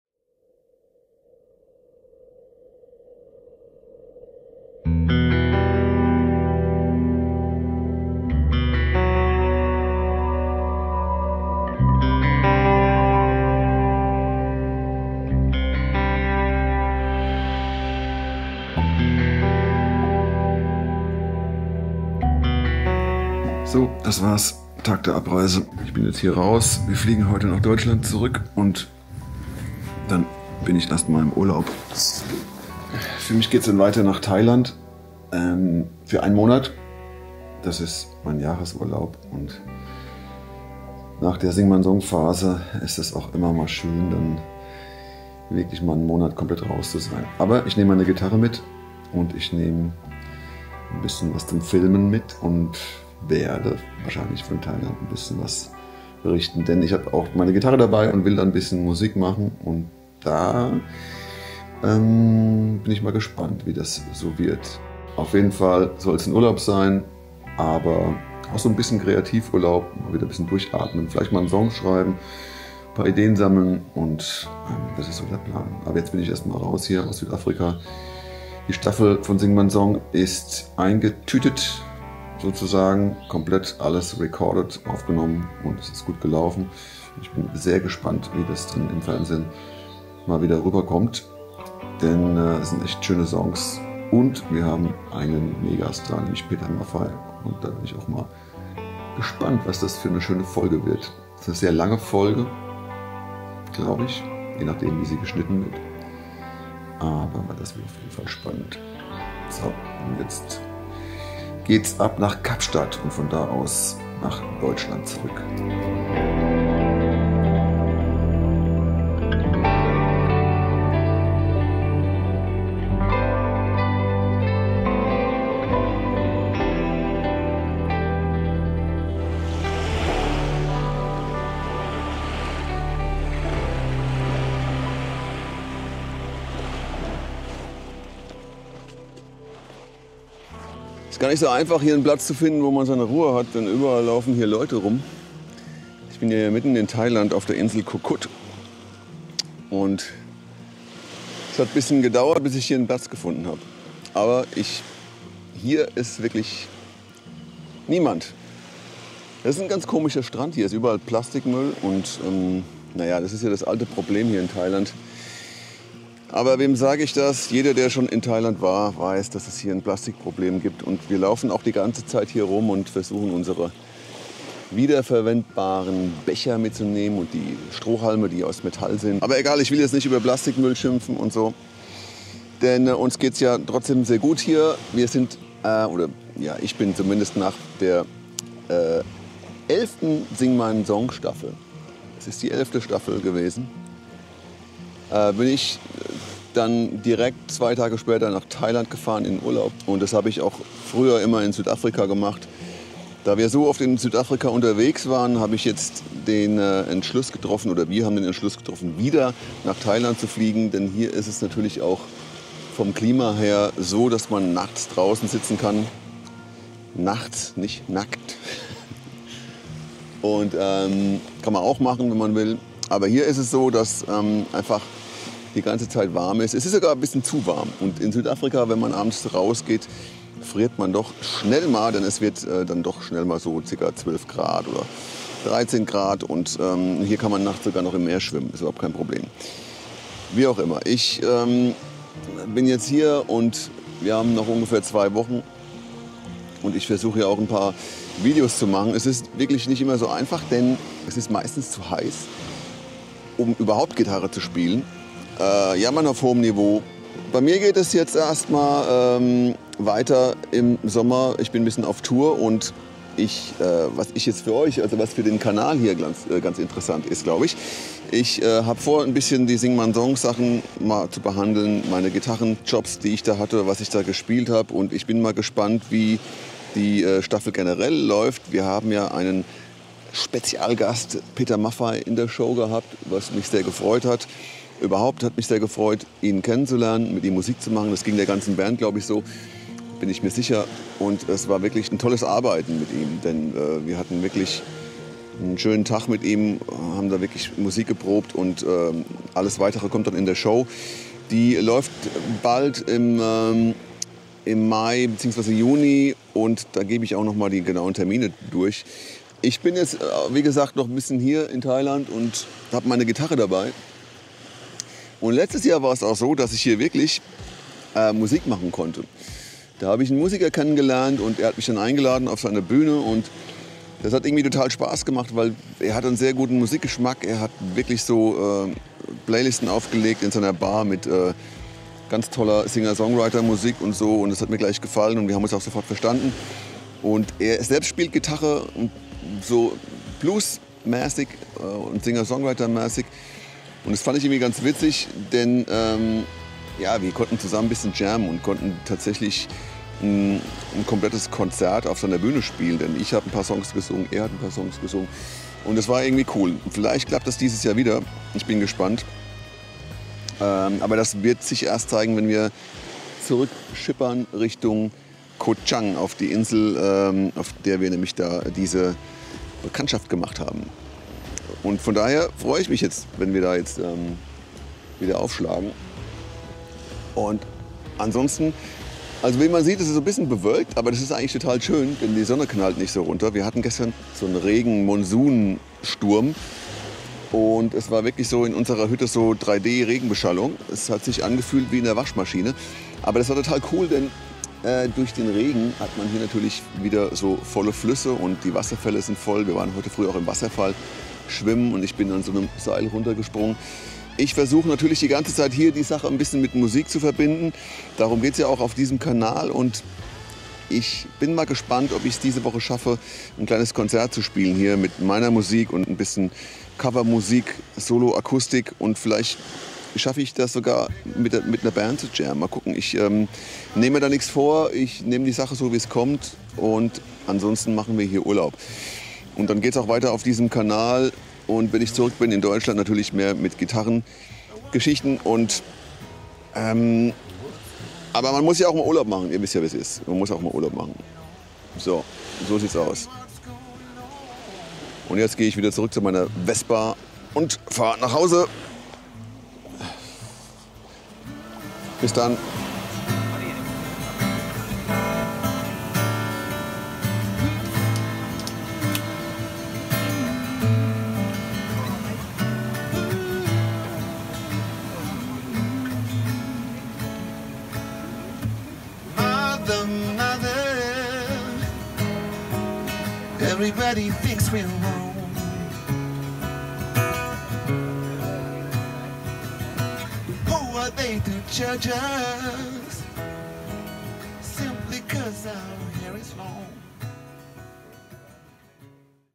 MVLOG#22 - SING MEINEN SONG ist geschafft. Wie geht es weiter?
VON DER TV-SHOW IN DEN URLAUB (mit Gitarre)! Von Südafrika nach Thailand